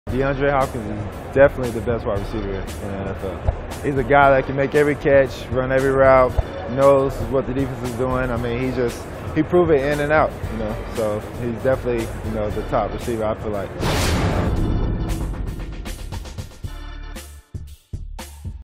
Deshaun Watson thinks DeAndre Hopkins is 'definitely' the best WR in the NFL | FOX NFL
Nothing but praise from one Texans star to another.